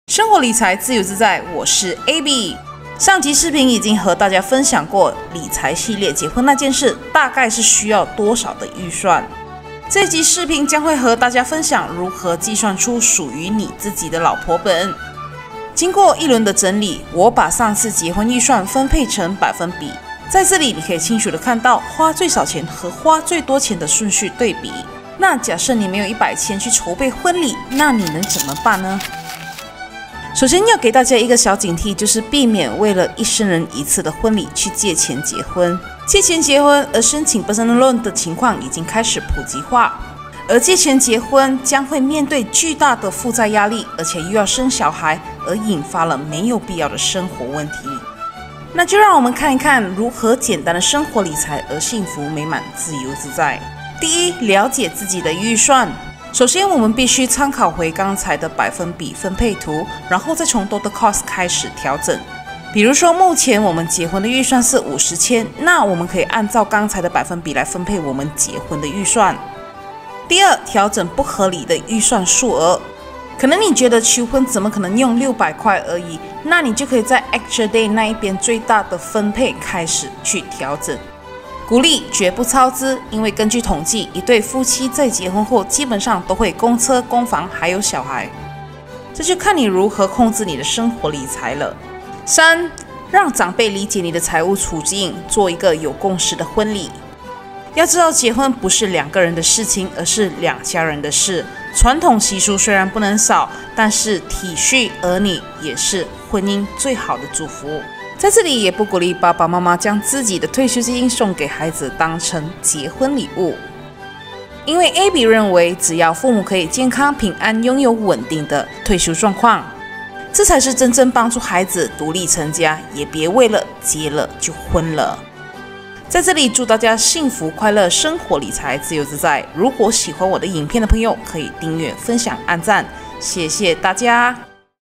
如何计算出属于你自己的老婆本⁉️⁉️
PART 2: 如何计算出属于你自己的老婆本⁉️⁉️

1️⃣如何用百分比分配法来了解自己的预算🤩
2️⃣调整不合理的预算数额，但是绝不超支😎
3️⃣让长辈了解你的财务处境，有共识的结婚💒
娶老婆不用怕，一切从设计属于你们婚礼的“老婆本”开始，
不想结了就“昏"了，记得看看这个视频！

请记得帮我点击，按赞，分享。